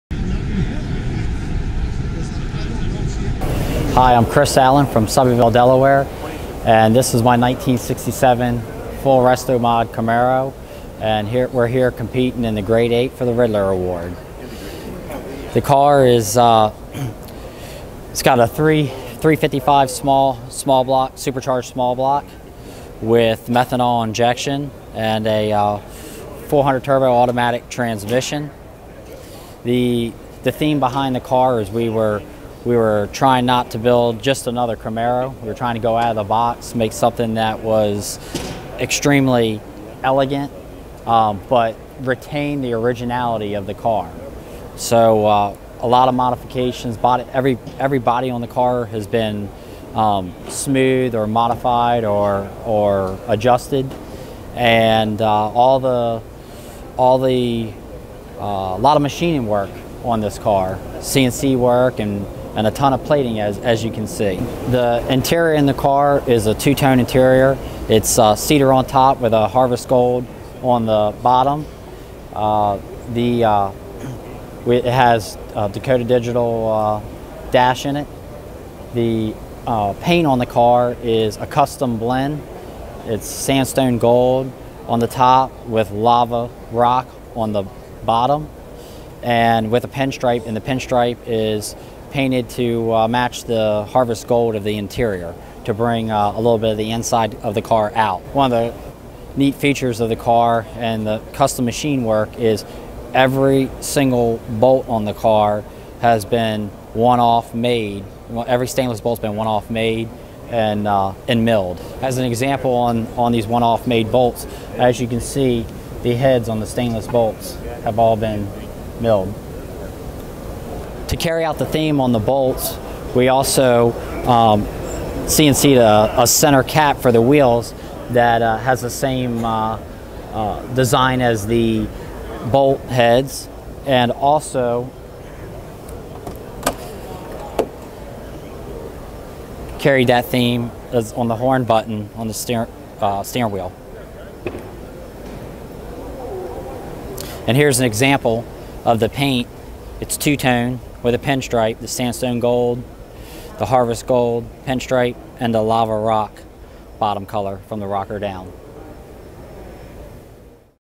1967 Camaro
The 1967 Camaro Street Machine is owned by Chris & Greg Allen of Selbyville, Delaware. On Friday, it was selected as a Great 8 Award winner and eligible to win the prestigious Ridler Award here at the Detroit Autorama for 2018.